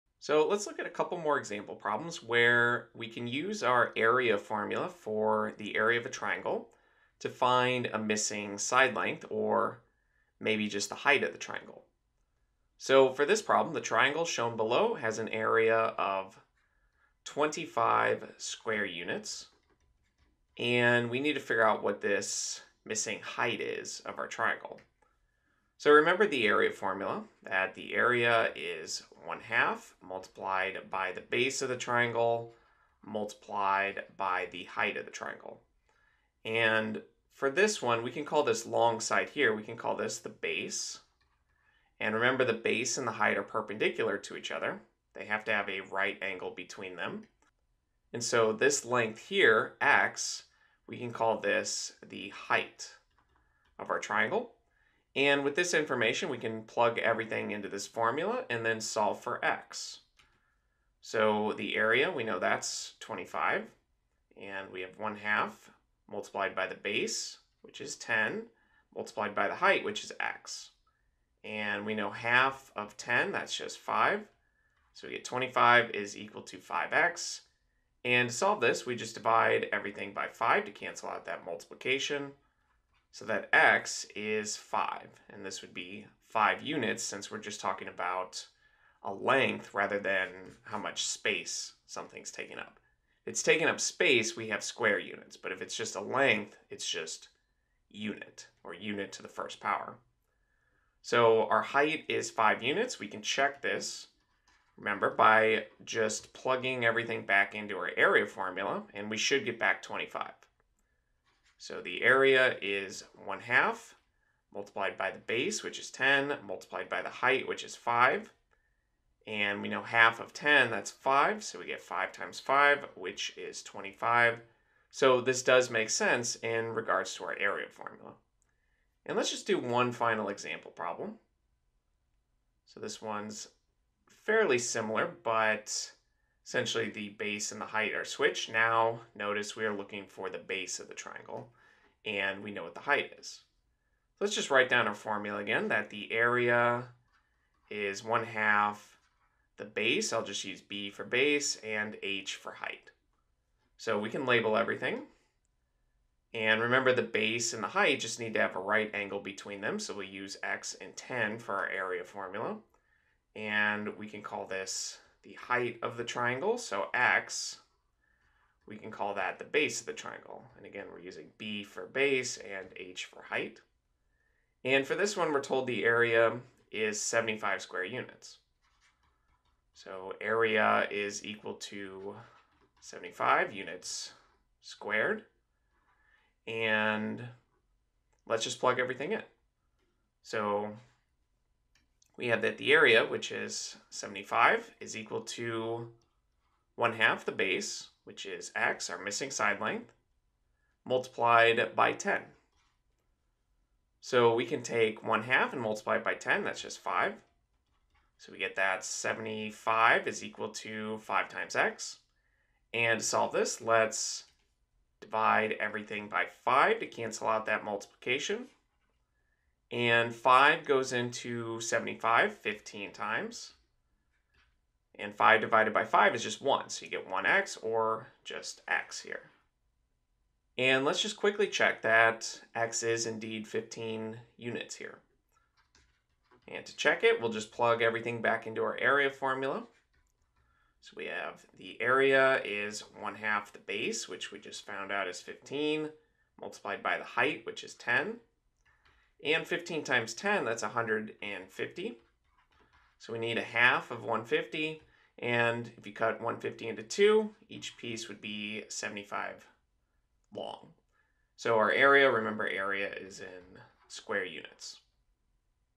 Area of a Triangle: Missing Side Length (Practice)
This video continues with the concept of using the formula for area of triangles to find a missing side length or height of a triangle.

The strategy for these problems is to set up an equation using the area formula and the missing side length or height. Once the equation is solved and an answer is found, the result can be checked by plugging the base and the height back into the area formula to make sure that it matches with the given area.

The general formula for finding the area of a triangle is to multiply the base of the triangle by the height and divide by 2. Triangles take up exactly half the area of a rectangle that can be formed around it.

Khan Academy exercise to practice:
1. Find Missing Length When Given the Area of a Triangle

EulersAcademy.org